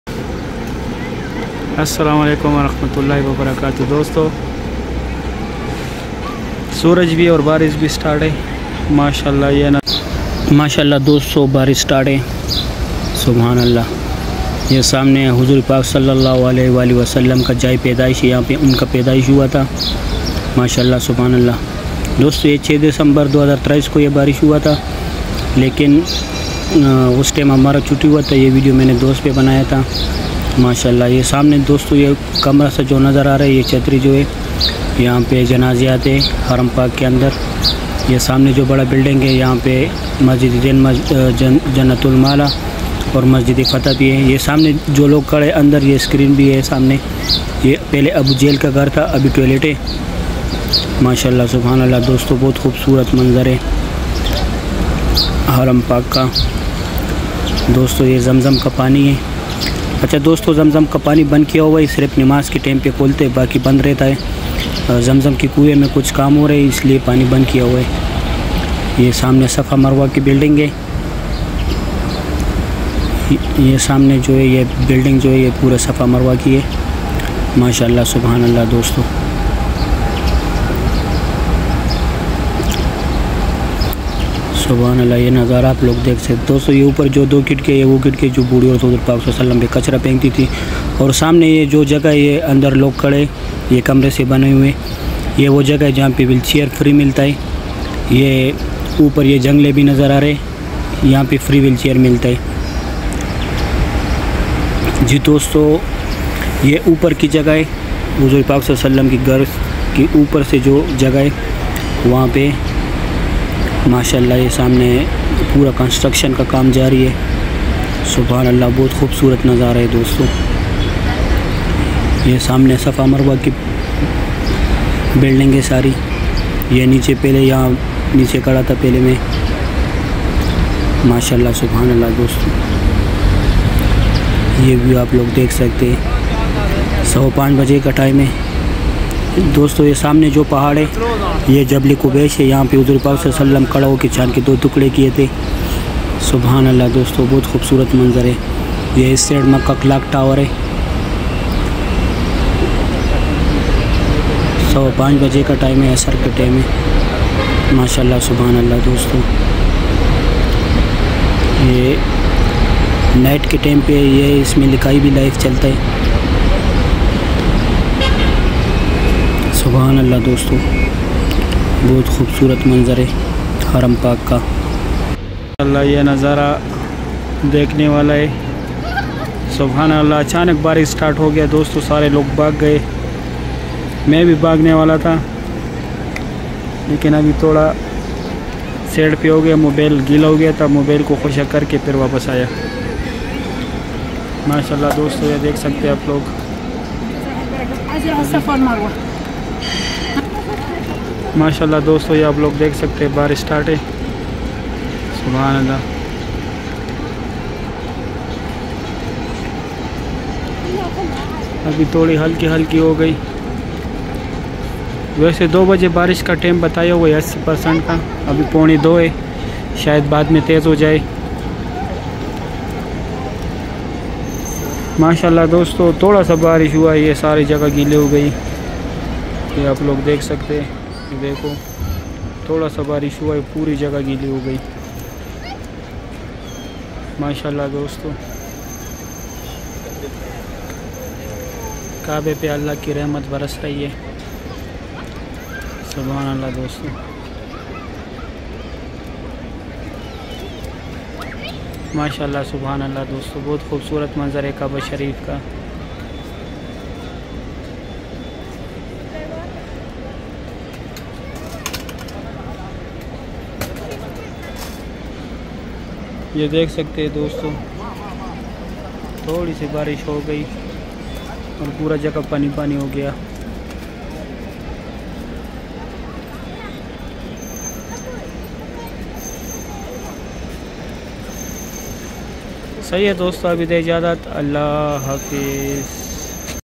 Makkah haram sharif | 6 December 2023 | kaaba live🔴 | beautiful Rain☔🌧 in Makakh | Makakh official
🔴 Today  live MAKKAH Haram Tawaf e Kaaba 🕋
The holy tawaf e kaaba